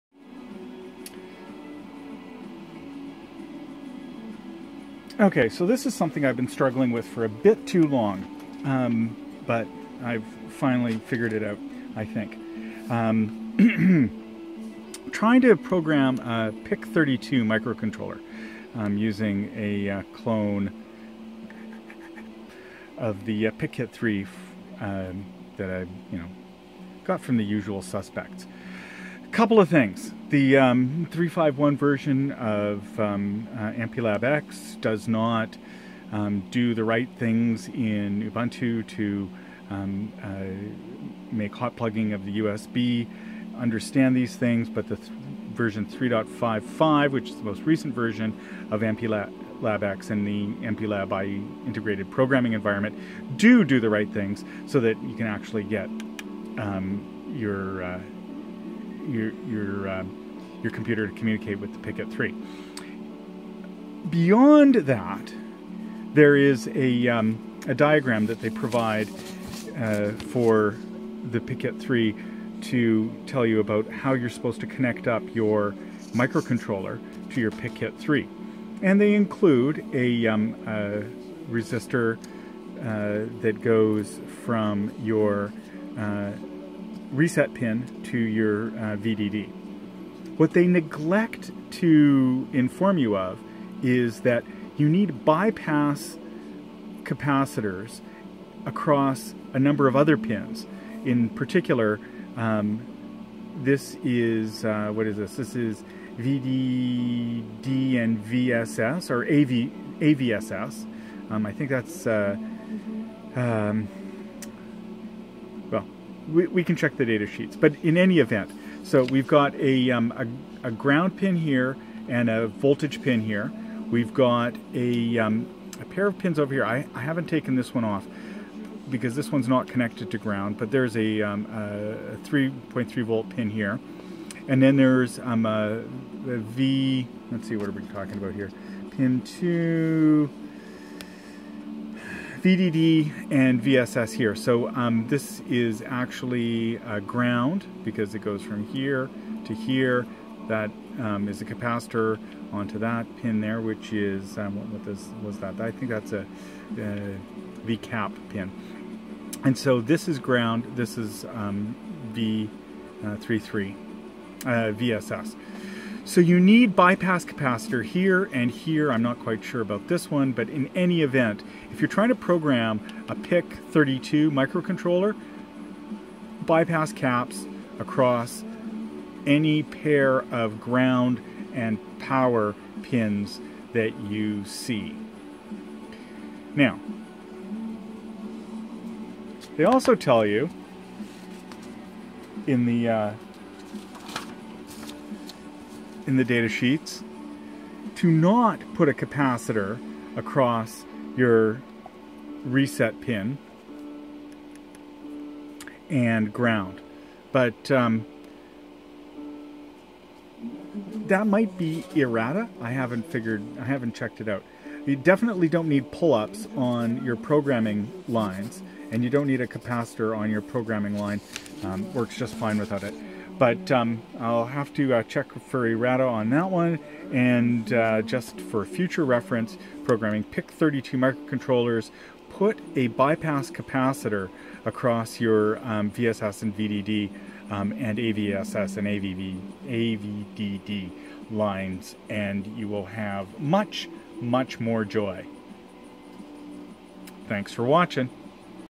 Programming a PIC32 notes
A quick note so that I remember what is needed to program fast PIC microcontrolers.  In particular I am trying to program a PIC32MX250F128B.  I needed to add bypass caps across Vss/Vdd and AVss/AVdd (.1uF worked).

And!  The cap across Vss and Vcap (pins 19 and 20 on the sdip) IS needed.

I was doing this so that I could build another oscilloscope.  Trying to be like a Cornell EE student.